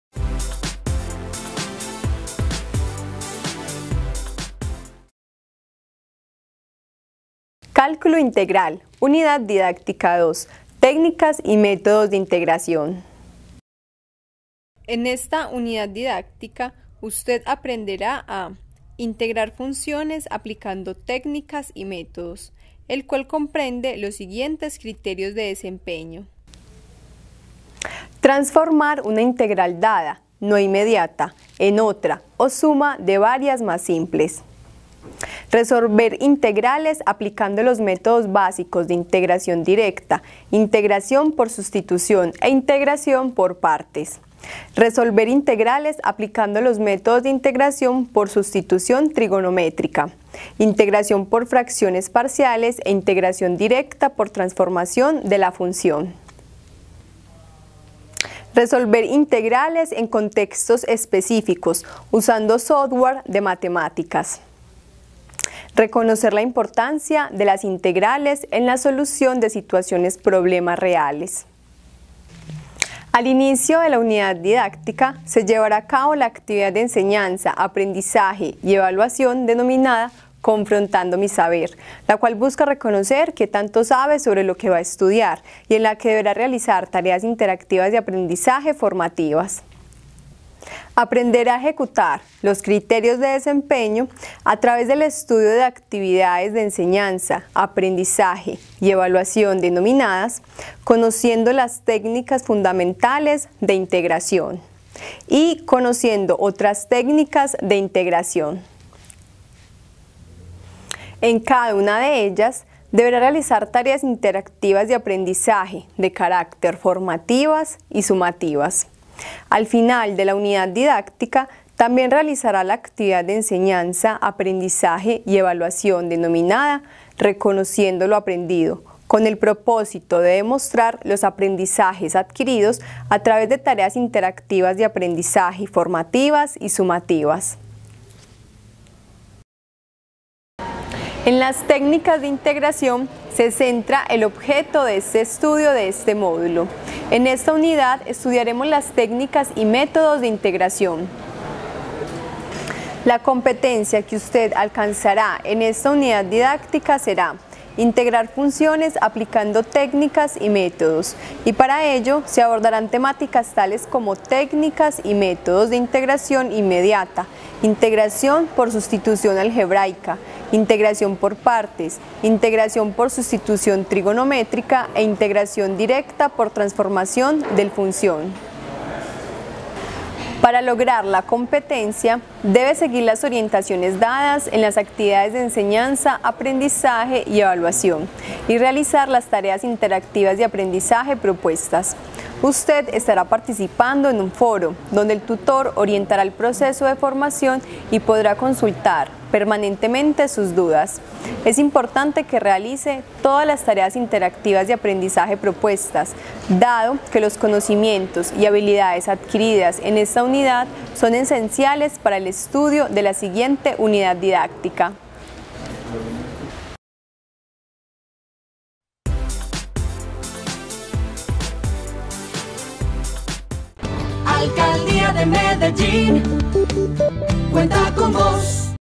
Cálculo Integral UD2 – Técnicas y Métodos de Integración
En esta unidad se estudiarán las antiderivadas interpretadas como el proceso inverso de las derivadas, con  ellas se pueden calcular velocidades y posiciones de cuerpos o partículas conociendo la función de la aceleración de dicha partícula o de dicho cuerpo. La antiderivada de una función es conocida como la primitiva de la función y es el Teorema fundamental del Cálculo el cual relaciona las antiderivadas con las integrales.

Coordinador

Pedagogo

Asesores Instruccionales

Soporte Tecnológico

Diseño Gráfico

Producción Multimedial